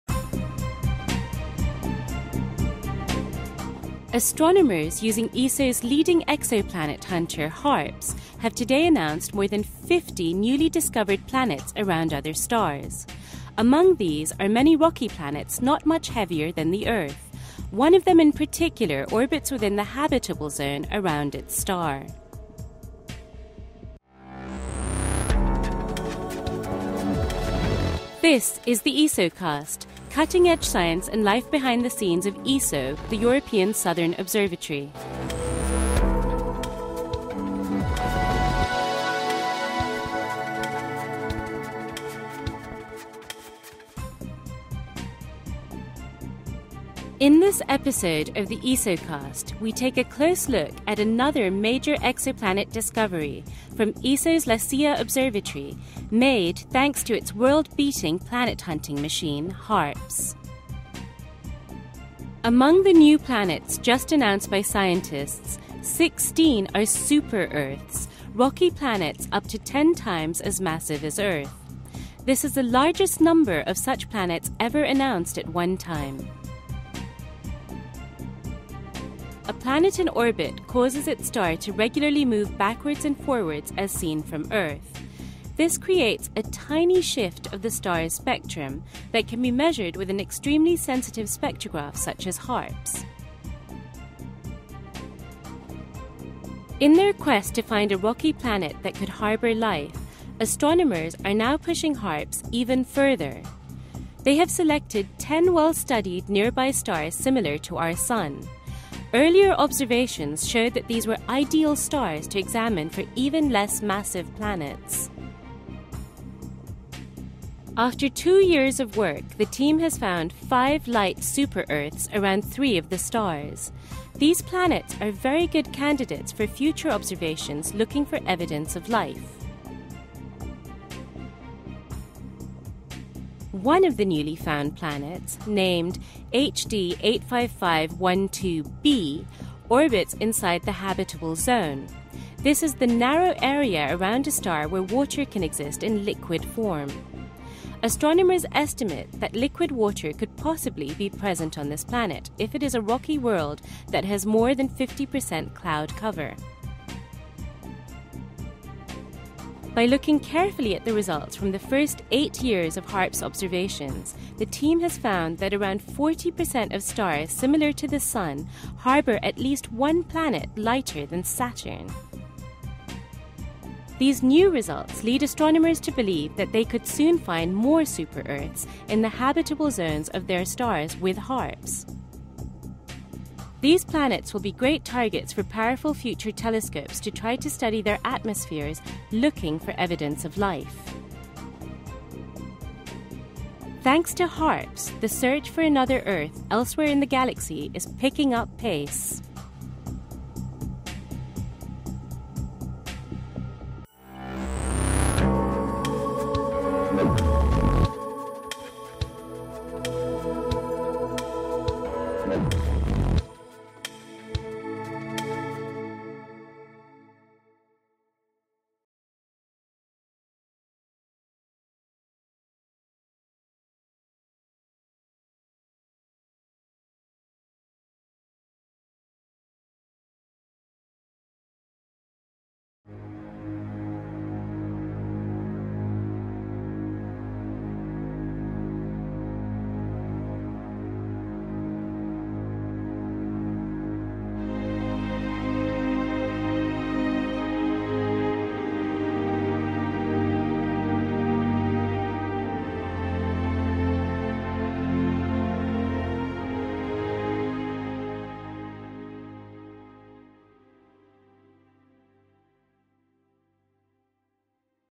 16 New Super-Earths Just Discovered
Astronomers using Europe's HARPS telescope have discovered 50 new exoplanets orbiting nearby stars - including these 16 super-Earths, one of which lies within its star's habitable zone.